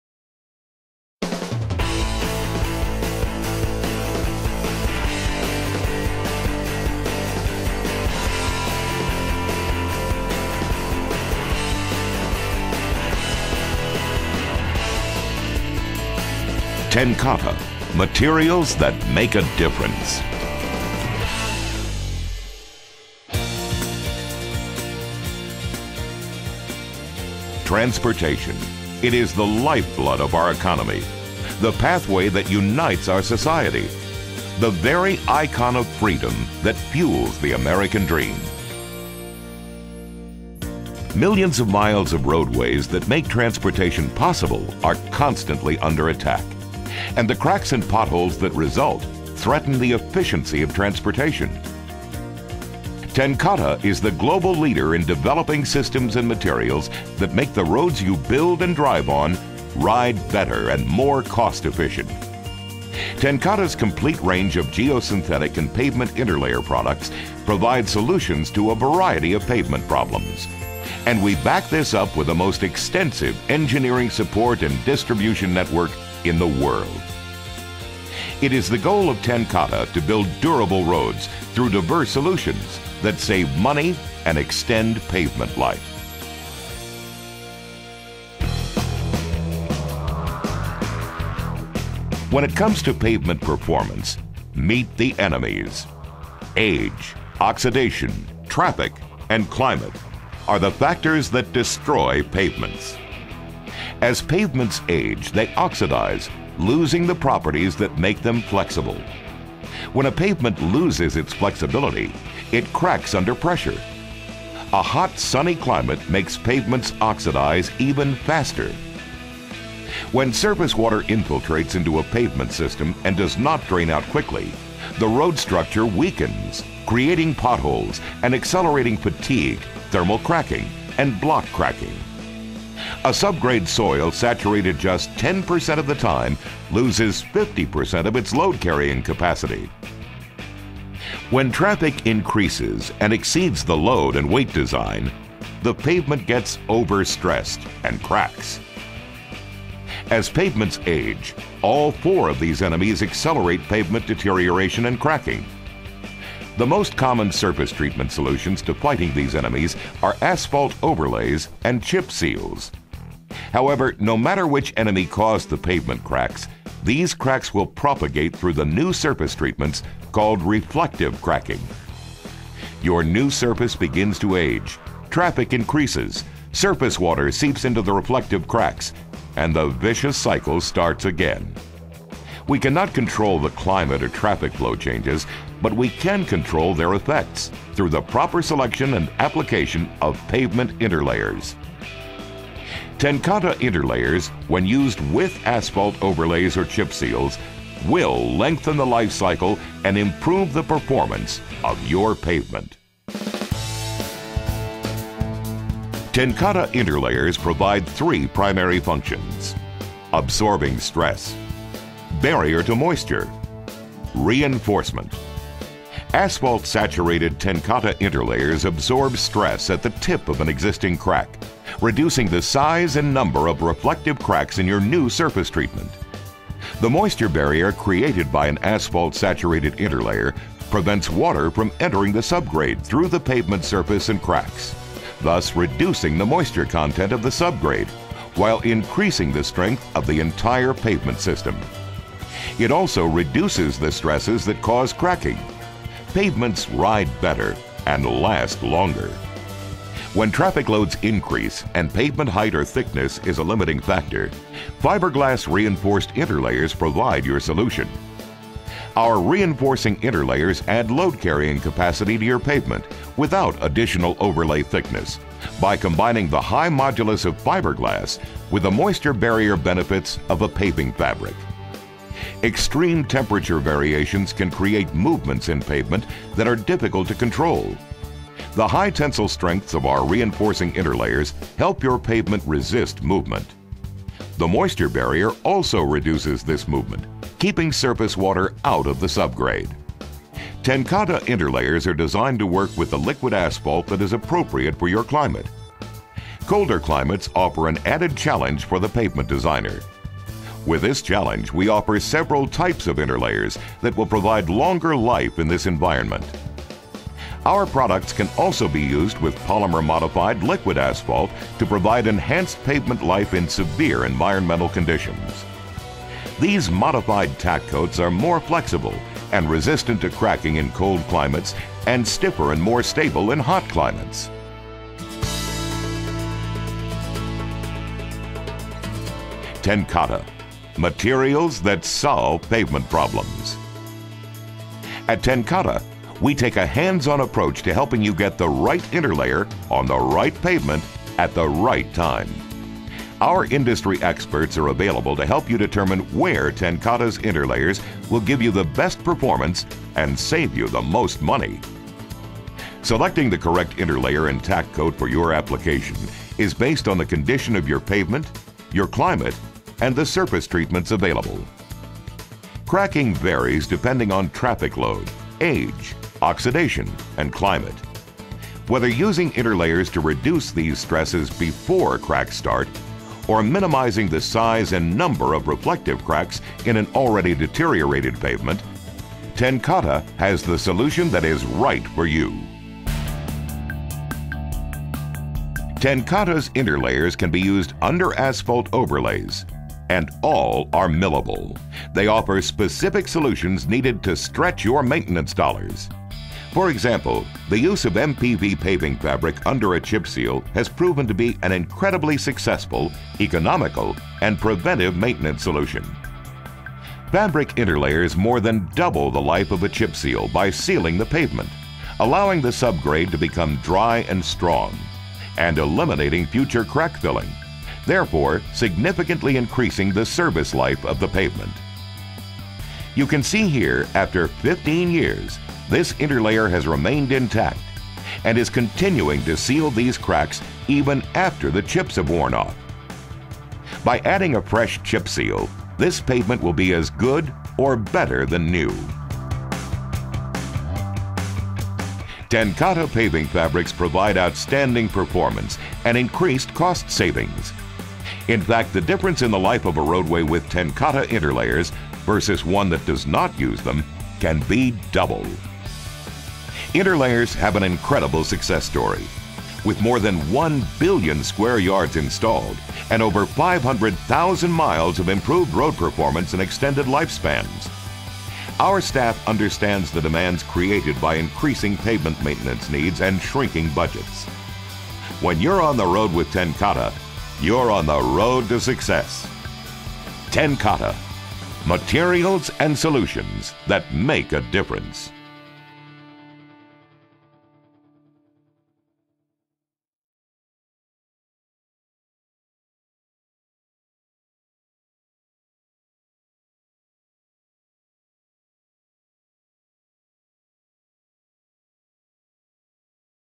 Pavement Solutions
TenCate Geosynthetics deliver solutions that enhance the performance and design life of transportation engineering structures for paving.  From our complete product line for Roadway and Railway Construction System Solutions, we offer materials that function for sealing, stress relief, adhesive bonding, and soil reinforcement.  They offer robust mechanical properties coupled with high water flow capabilities that extend pavement life by retarding reflective cracking, waterproofing and reinforcing asphalt pavements.